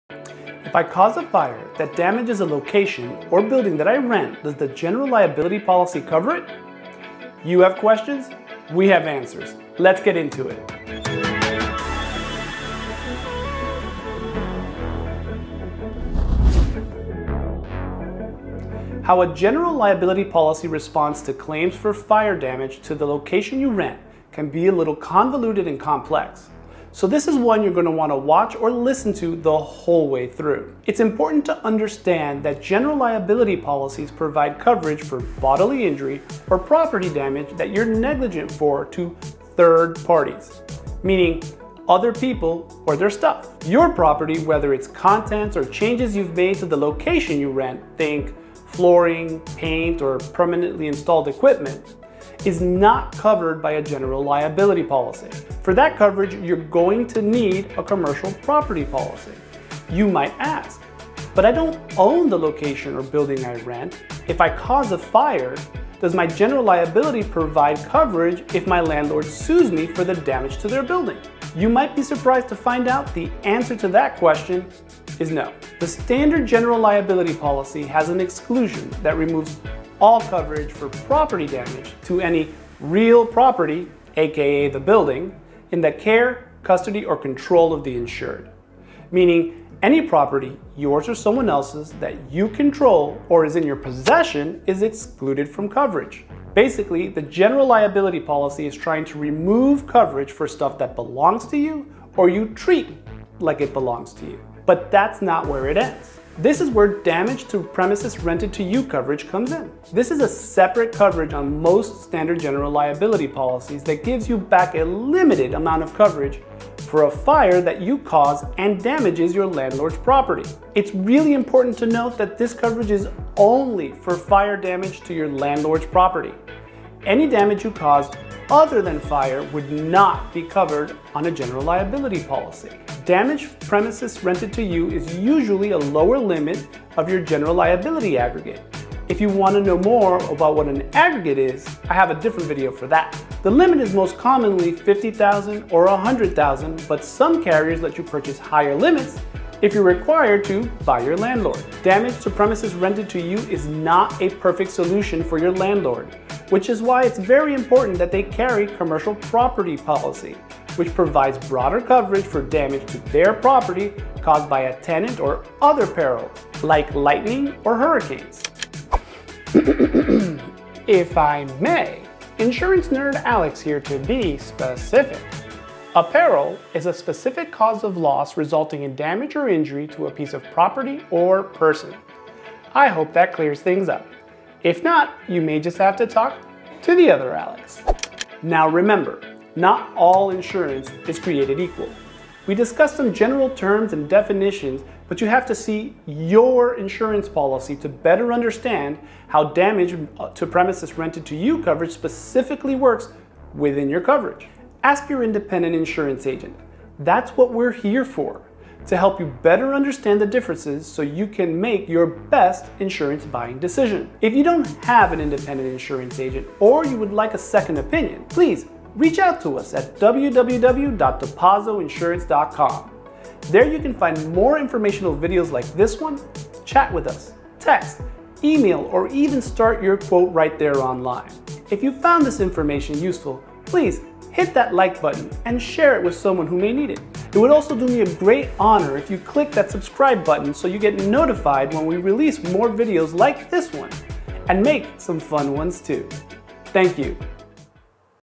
Does General Liability Cover Damage To Rented Premises?
If you cause a fire that damages a commercial location you rent, does your general liability policy cover it? The answer is a bit more complicated than you might think.

In today's video we'll take a look at different scenarios and explain why or why not you'll be covered.